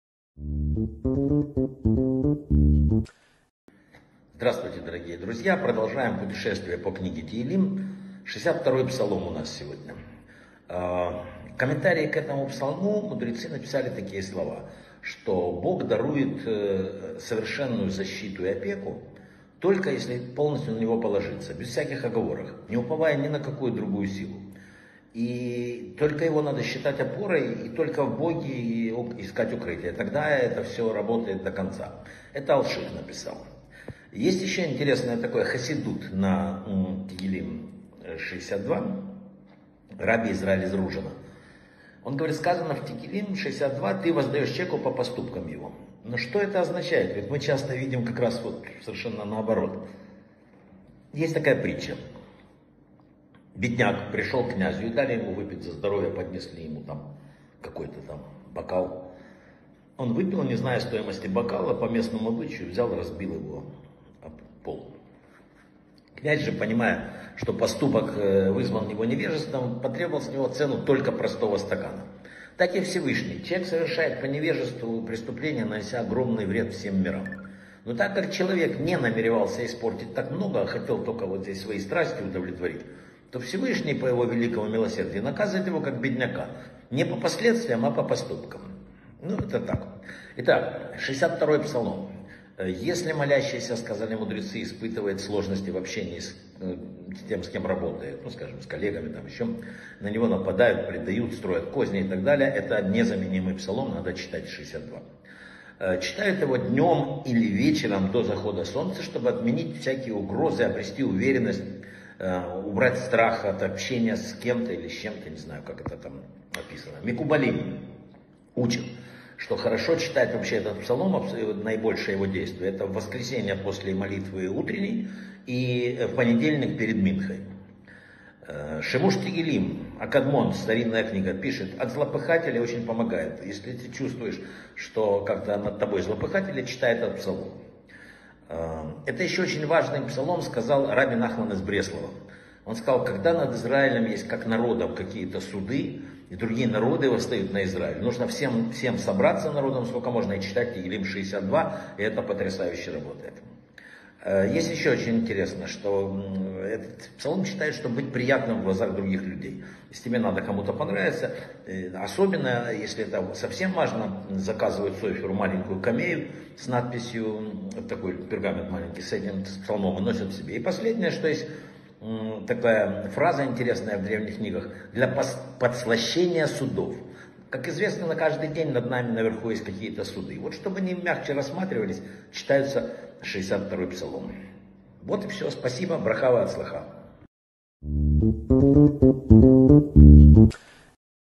📗 Псалмы Давида. Псалом 62. Положись на Бога безоговорочно. Уверенность в общении | Вадим Рабинович
Шестьдесят второй Псалом - что пишут в комментарии к нему мудрецы о том, кому Вс-вышний дарует Свою защиту и опеку?
Хасидский комментарий раби Исроэля из Ружина на слова этого Псалма - "Ты воздаешь человеку по поступкам его". Что это означает?
Притча о бедняке, который пришел на трапезу к князю и разбил дорогой бокал по обычаю, не зная его стоимости. Какая здесь параллель с тем, как поступает с нами по Своему милосердию Вс-вышний?
Решение проблем при возникновении проблем в общении с коллегами на работе.
Как следует читать этот Псалом, чтобы убрать стрях, обрести уверенность в общении и отменить тревожащие человека угрозы?
Как помогает этот Псалом от злопыхателей? Какое его особое свойство подчеркивал раби Нахман из Бреслава для всего Израиля?
Обретение приятности в глазах других людей. Что нужно сделать, если предстоит какая-то особо важная встреча или знакомство?
Помощь этого Псалма для подслащения судов на Небесах.